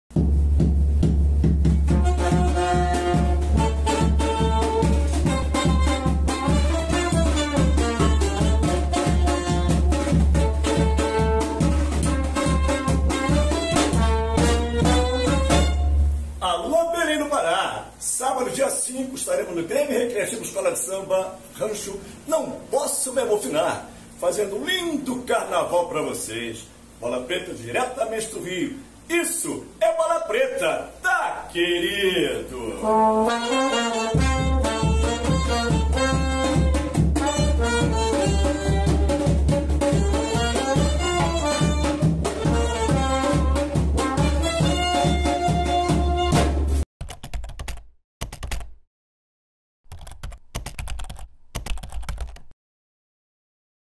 Cordão do Bola Preta se apresenta em Belém
Integrantes do tradicional bloco do Rio de Janeiro convidam público para folia em Belém